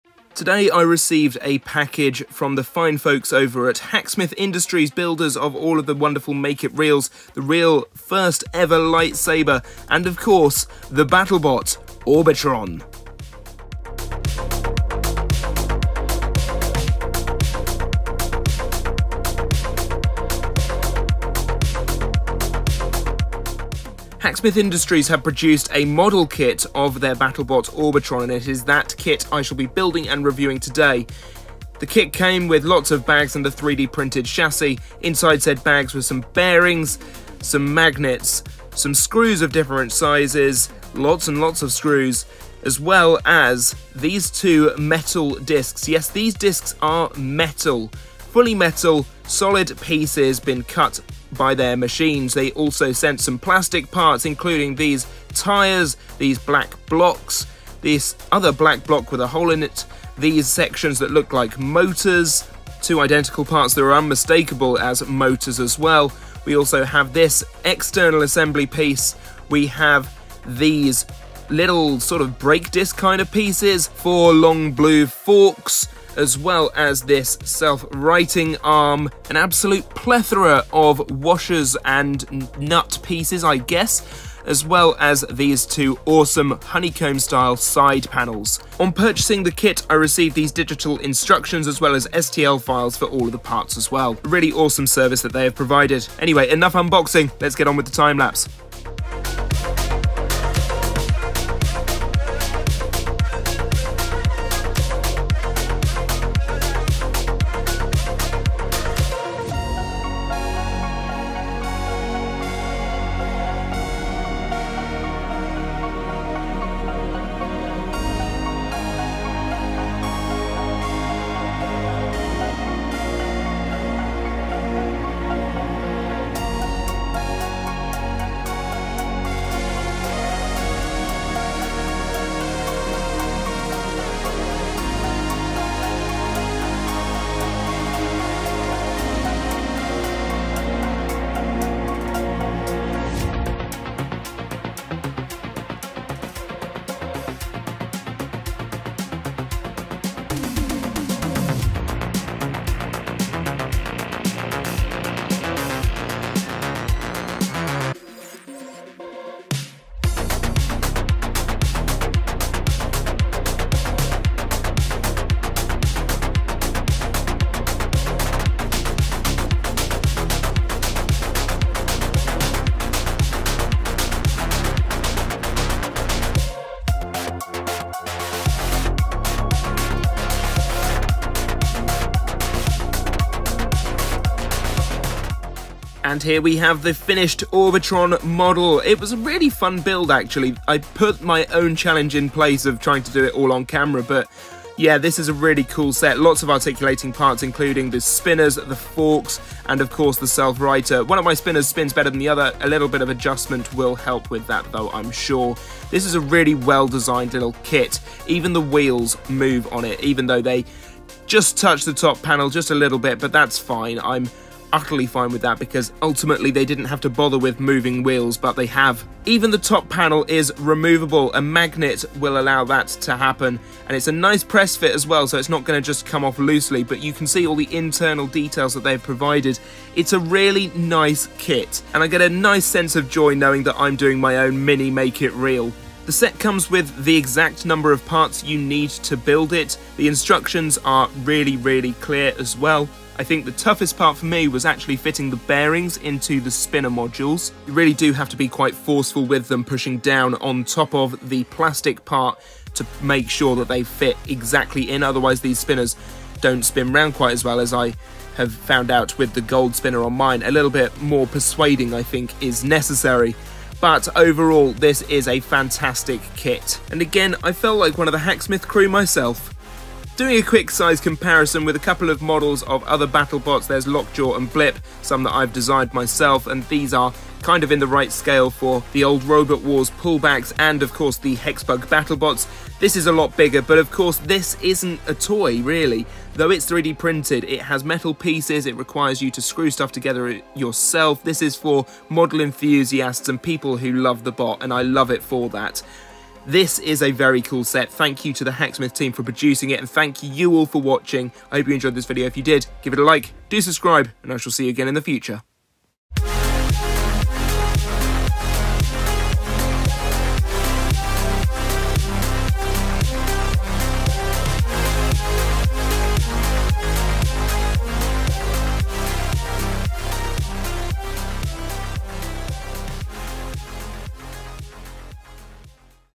Orbitron BattleBot Model Kit Review
Hacksmith Industries produced a limited run of merchandise for their BattleBot Orbitron earlier this year, and I was lucky enough to get my hands on a very nice piece, the scale model kit of the bit itself! Join me as I  unbox all of the parts, time lapse build the model and give my thoughts on the kit altogether!